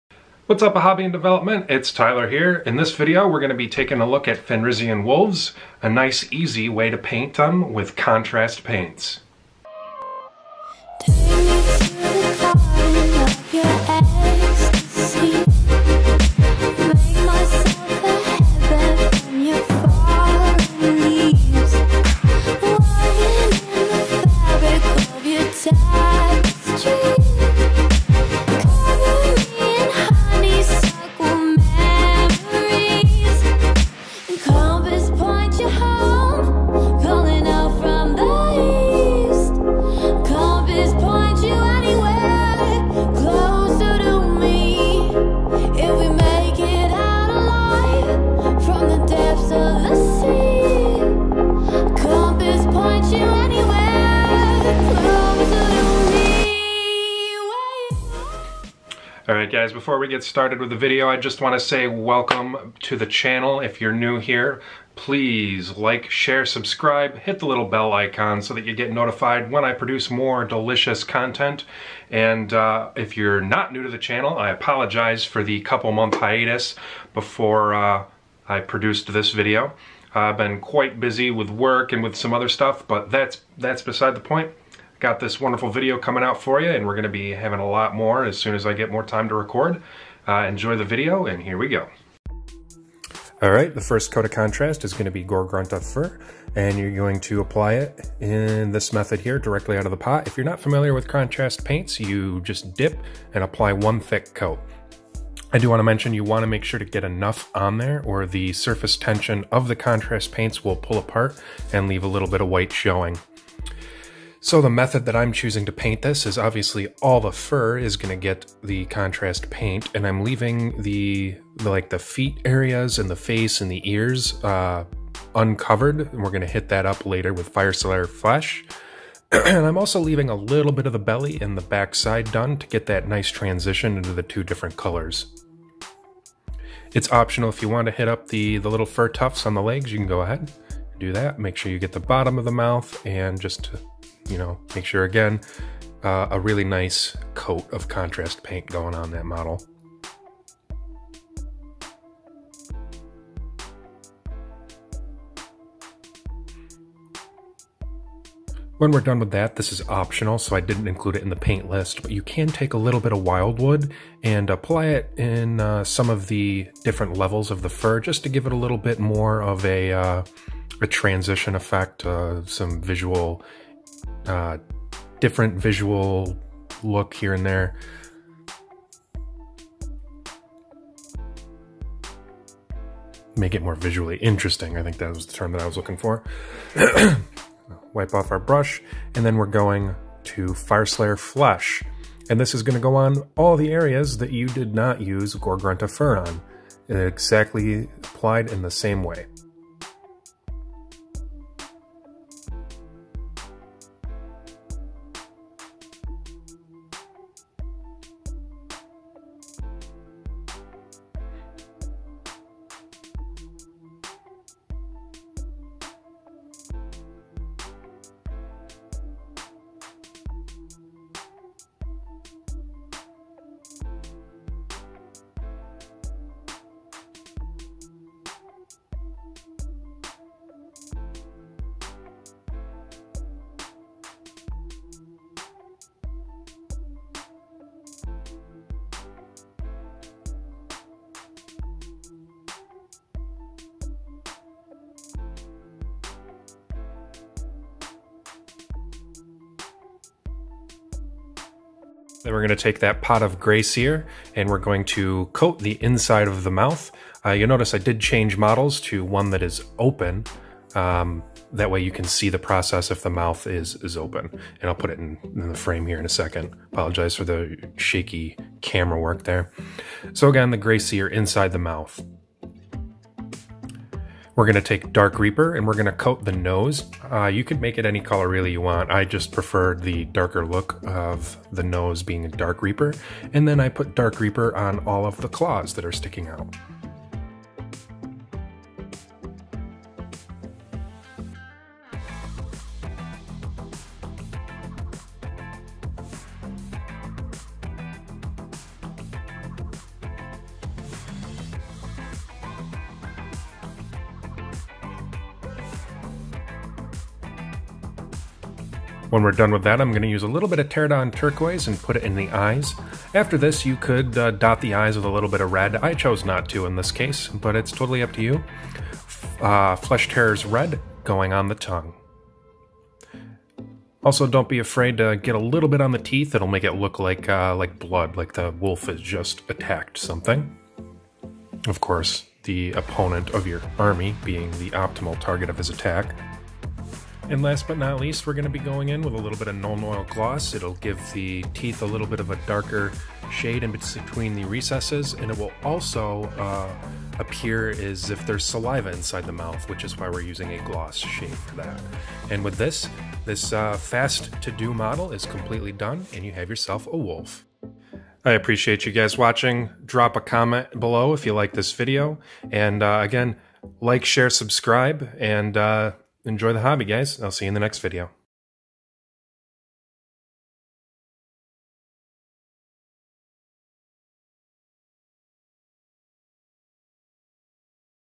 How to paint Fenrisian Wolves with Contrast Paints
Learn how to paint Space Wolves Fenrisian Wolves with Contrast Paints. Lets make these space puppies look fantastic!